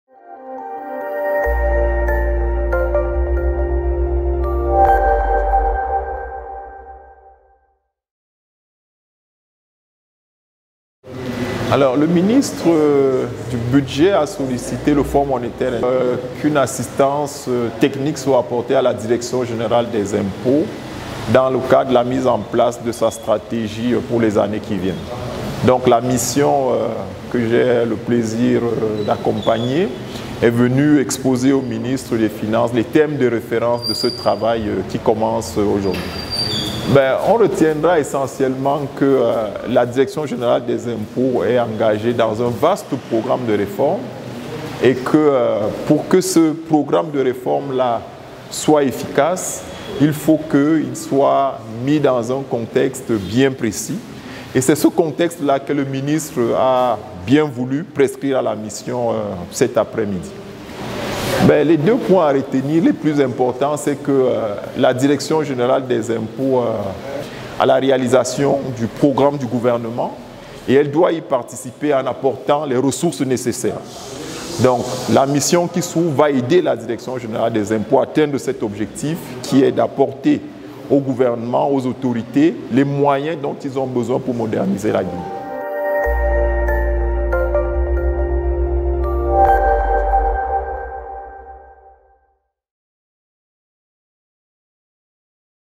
M. Nicolas HIOL du FMI : Les stratégies fiscales pour le développement de l'Afrique de l'Ouest
M. Nicolas HIOL du FMI a discuté des stratégies fiscales pour le développement de l'Afrique de l'Ouest lors d'une réunion avec le ministre du Budget. La direction générale des impôts est engagée dans un vaste programme de réforme pour soutenir le programme gouvernemental et fournir les ressources nécessaires à la modernisation de la Guinée. Une assistance technique sera apportée à la Direction Générale des Impôts pour atteindre cet objectif.